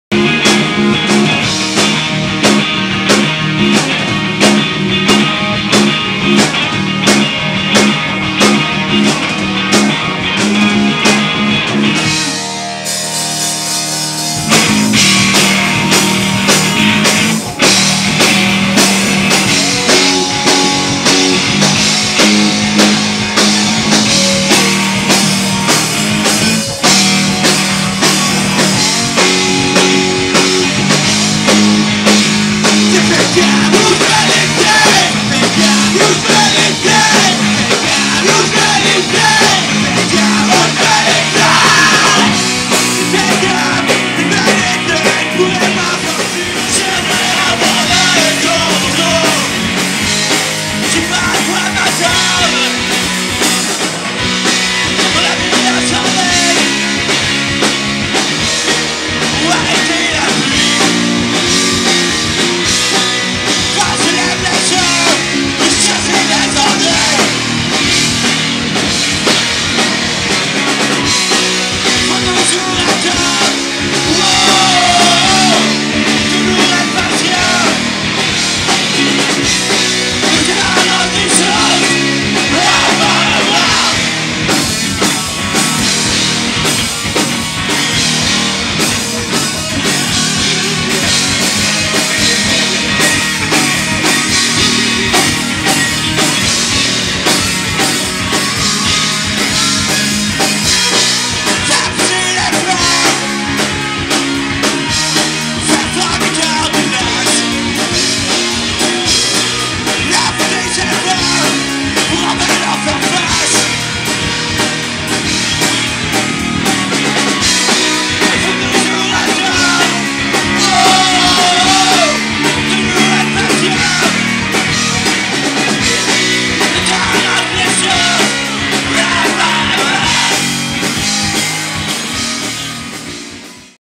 Le Monsieur - Soirée Barbier Records 8/12/12
Le Monsieur à l'Absynthe pendant la toute première soirée Barbier Records du 8 décembre 2012!

Caméra: Pierre-Olivier Cadieux
Montage: Julien Amyot